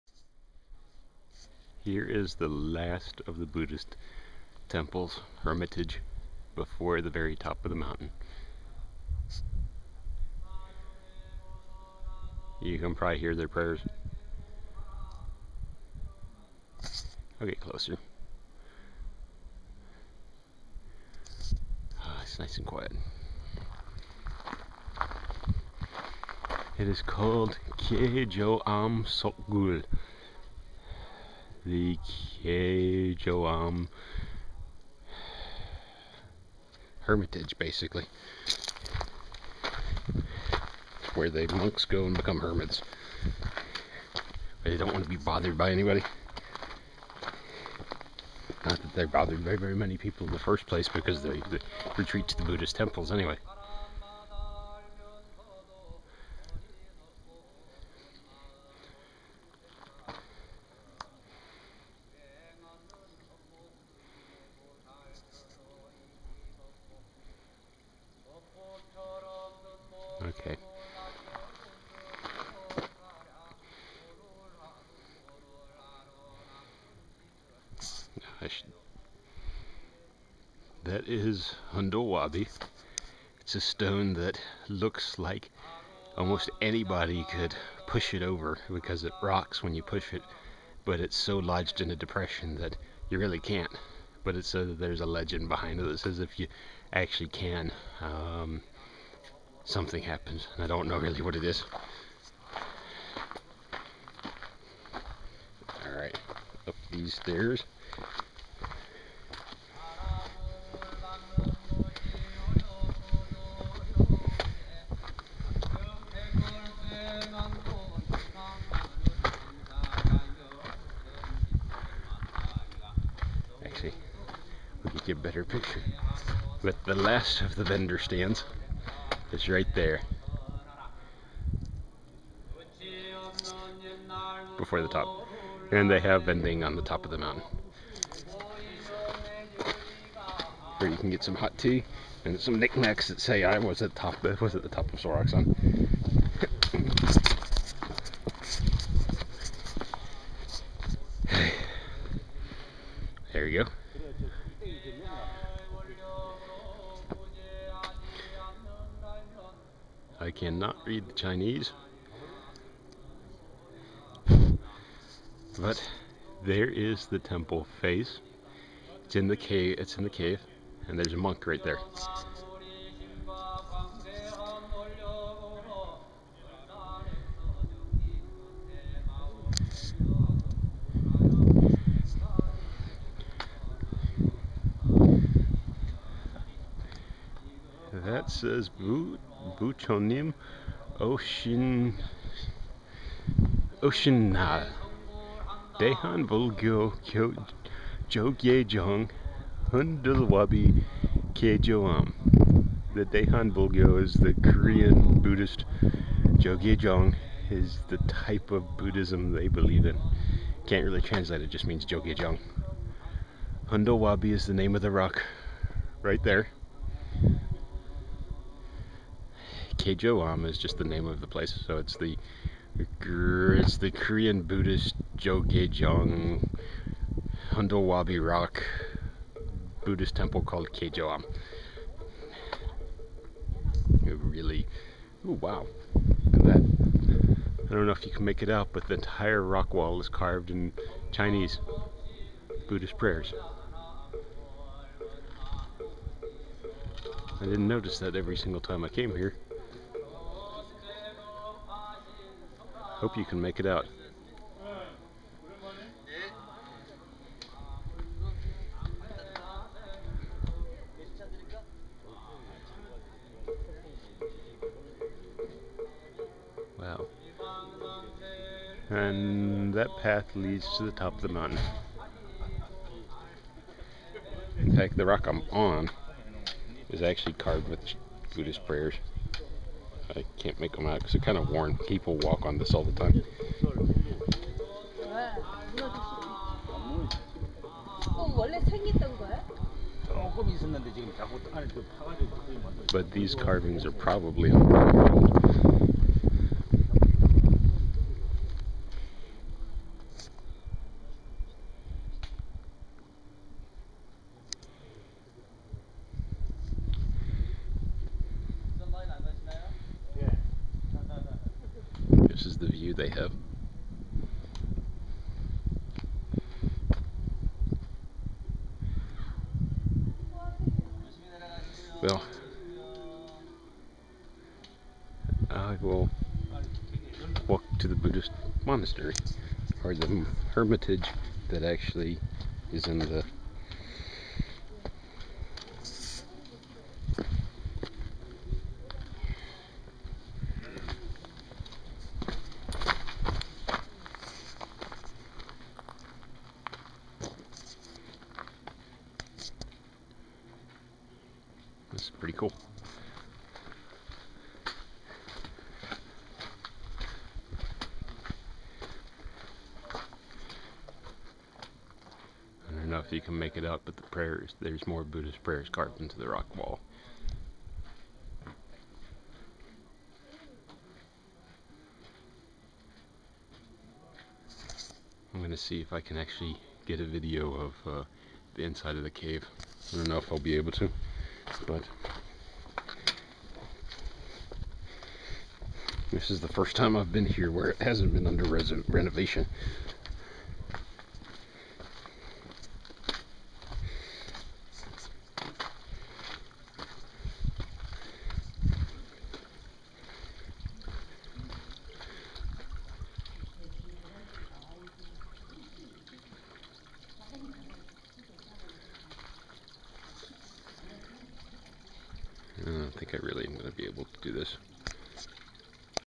Soraksan Buddhist hermitage
This is a buddhist hermitage near the tree line on Soraksan.  This is where the local buddhist monks go to get away from everything and meditate. Originally filmed in 2010.